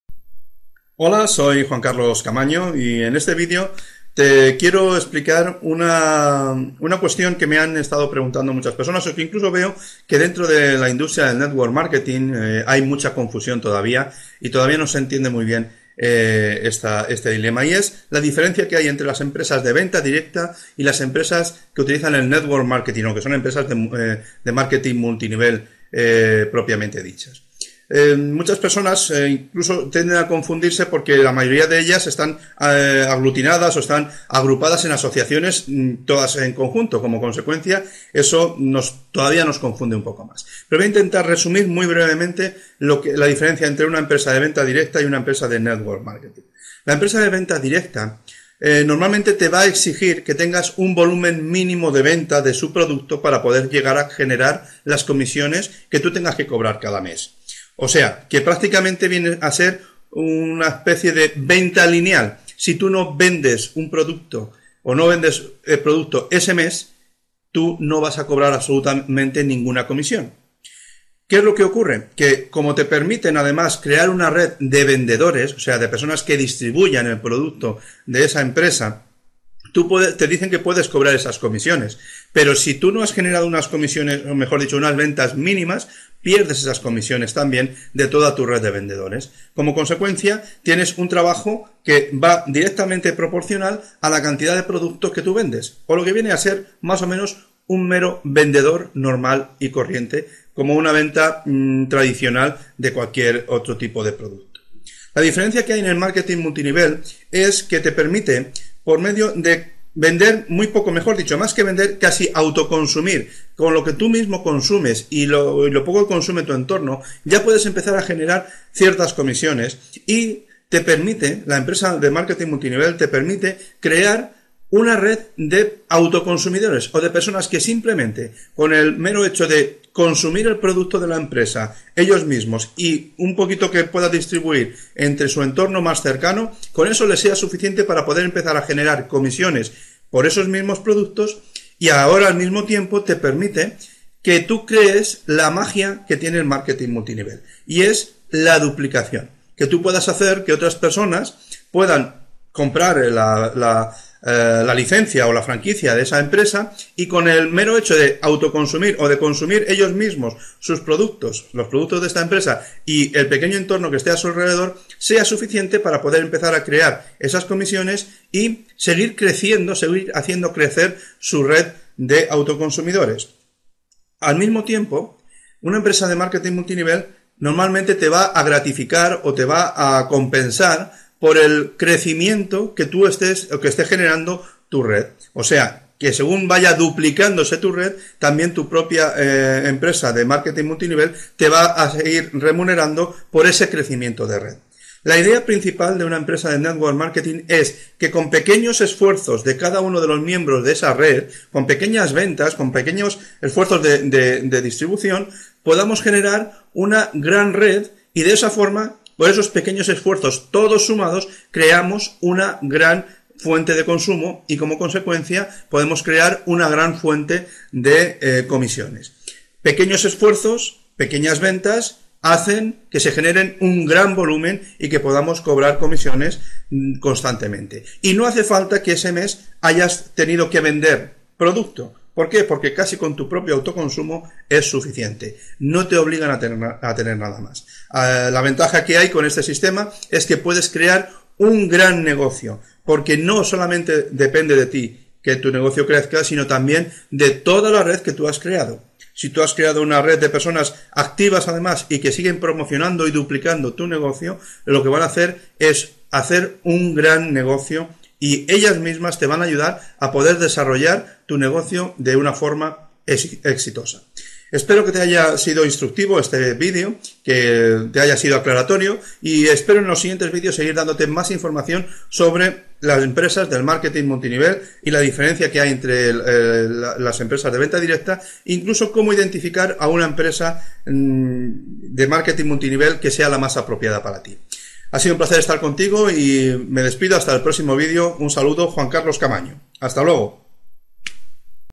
Diferencia entre VENTA DIRECTA Y Network Marketing
Una diferencia entre Venta directa y Network Marketing por Carlos Camaño